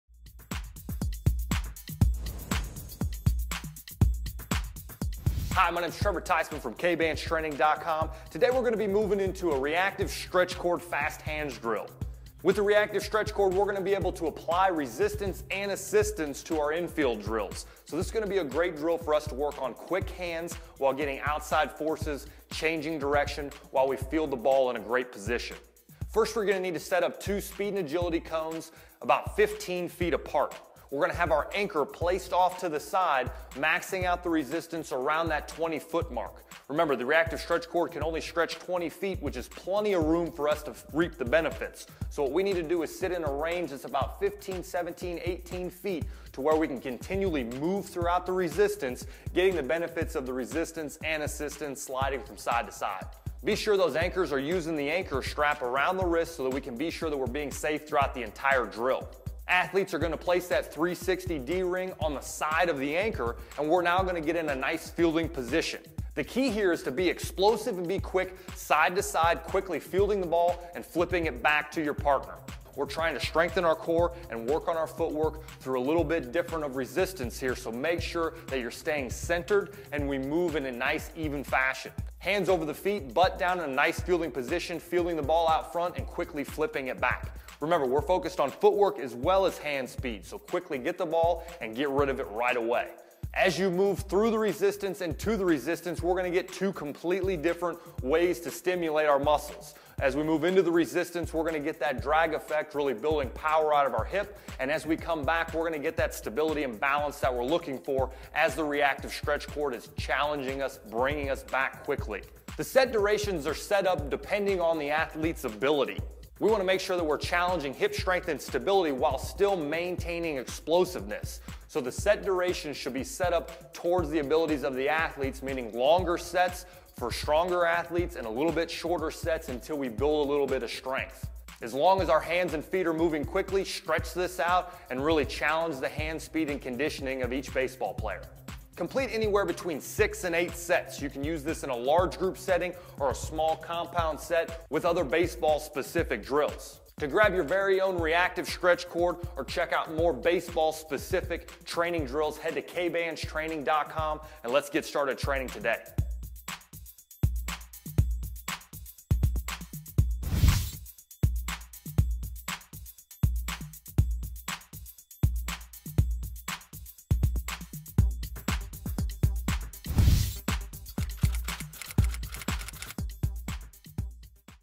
Baseball RSC Hands Drill | Indoor Baseball Drill
The RSC Fast Hands Drill is an excellent indoor baseball drill for developing fast twitch muscle fibers and making neurological connections between the brain and muscles. This allows for greater speed while performing tasks, like throwing a baseball, and increasing muscular power.

To complete the RSC Fast Hands Drill athletes will need the following: 2 Speed and Agility Cones, a Reactive Stretch Cord, 2-3 baseballs, a baseball glove, 2 partners (one to hold the Reactive Stretch Cord and one to throw grounders), and a space with at least 20 feet of straight unimpeded space.

One athlete will attach the Adjustable Belt around their waist while another partner will secure the Reactive Stretch Cord Anchor Strap around their wrist. The third partner will kneel ready to throw ground balls. After the Reactive Stretch Cord is attached athletes will place the 2 Speed and Agility Cones 15 feet apart while the partner with the anchor strap will stand 2-3 feet outside of one of the Speed and Agility Cones. The athlete attached to the D-Ring will start at one Speed and Agility Cone and begin to shuffle laterally toward the opposite Speed and Agility Cone. While moving laterally athletes will feel resistance from the Reactive Stretch Cord. As the athlete moves laterally, the partner with the baseballs will begin to throw grounders to the athlete moving between the Speed and Agility Cones. One baseball should be thrown as the athlete moves away from the anchor, while another grounder is thrown as the athlete moves back toward the original Speed and Agility Cone.

It is important athletes perform the indoor baseball drill under control and field and return the baseball as quickly as possible on every repetition. Sets of the RSC Fast Hands Drill will vary based on the athletes physical strength and maturity. If the athletes form begins to break down, the indoor baseball drill should be stopped and rest should be given. Perform 6-8 repetitions of the RSC Fast Hands Drill. Work this indoor baseball drill into small group training or mix this baseball drill with other baseball specific training. To see ideas and demonstrations of baseball specific drills head to KbandsTraining.com and explore the options in the baseball training section.